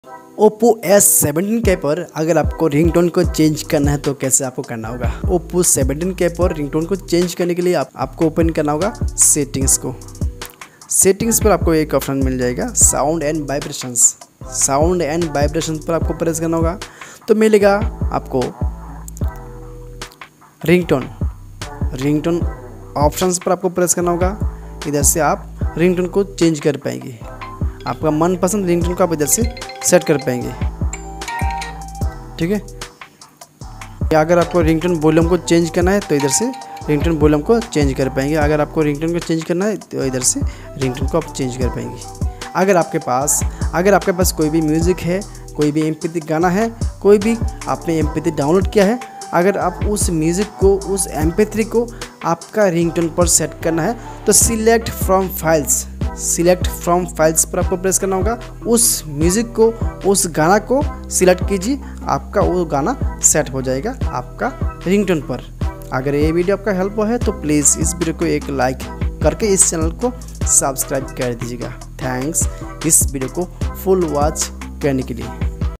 oppo a17k ringtone setting | oppo a17k ringtone kaise set kare | oppo a17k me ringtone kaise lagaye
Mobile Model Name : Oppo A17

Problem :  ringtone setting

Hariom Electronics
 India